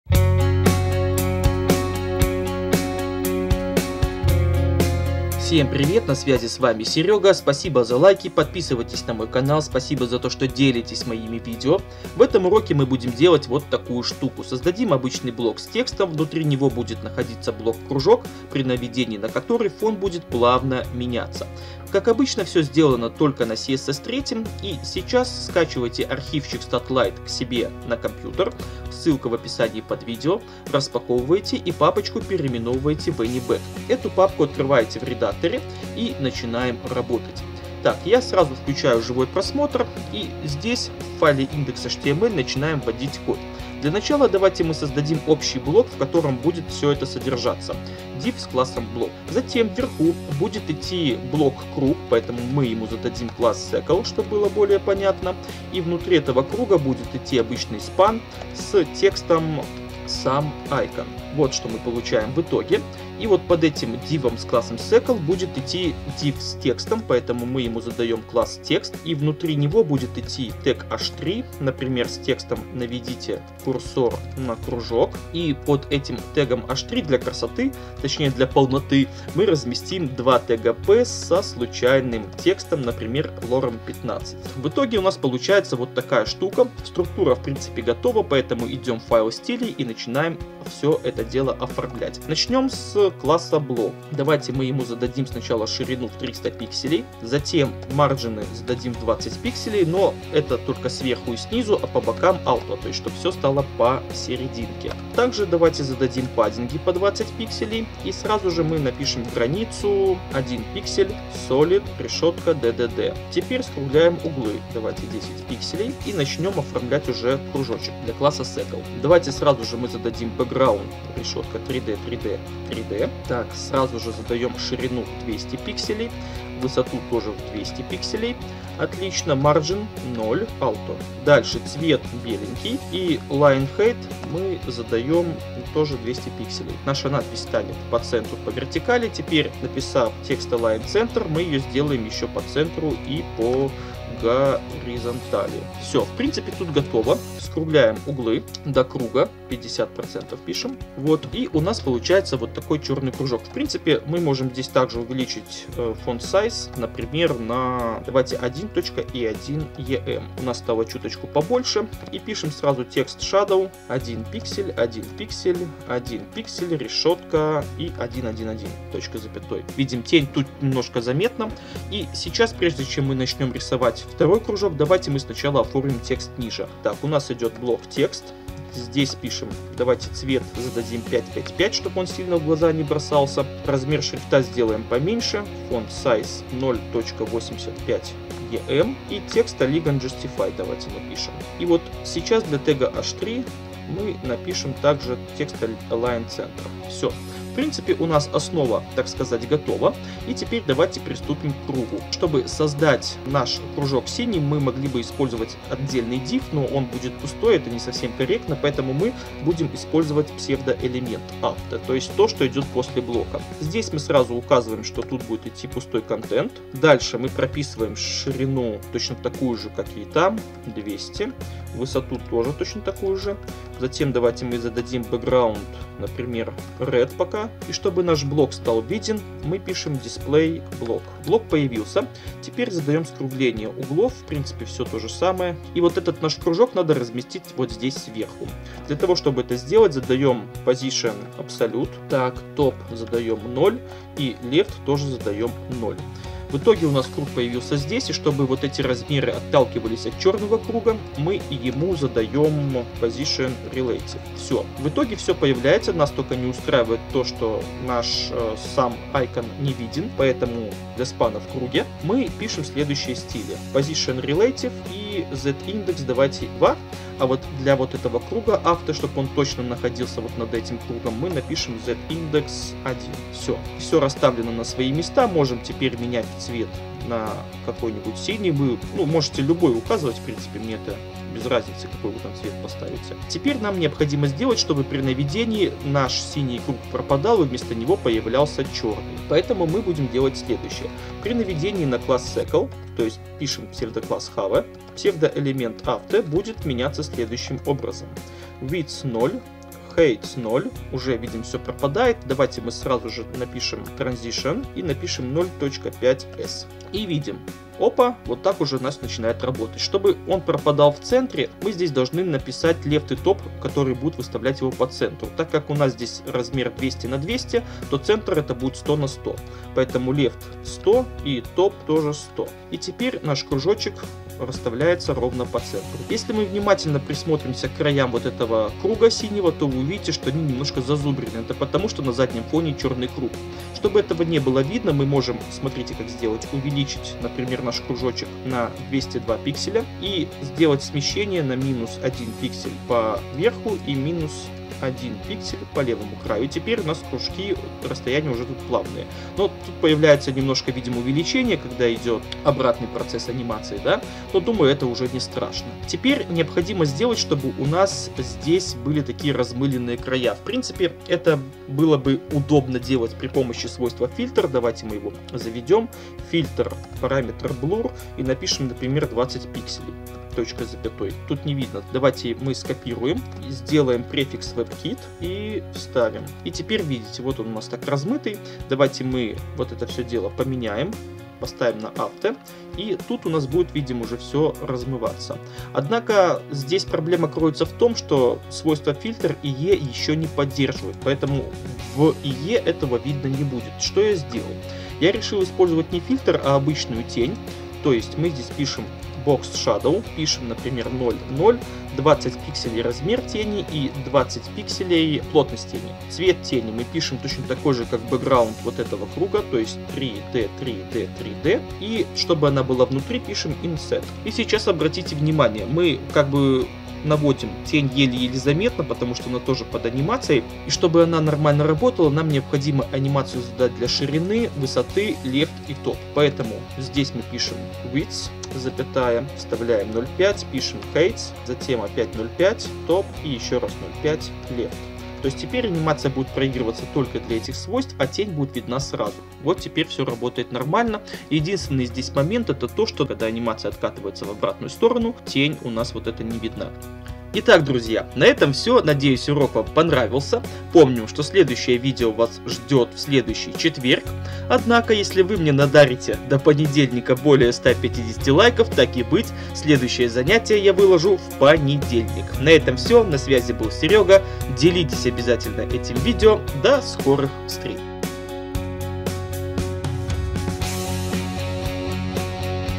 Красивая анимация фона при наведении
Следующее видео в четверг. Или... в ПОНЕДЕЛЬНИК! Если, конечно, наберём до понедельника 150 лайков :) Быть или не быть???

В этом уроке вы узнаете как сделать... блин, я даже не знаю как это описать. Лучше посмотрите на пример

Музыка предоставлена сайтом как партнеру VSP Group. Спасибо!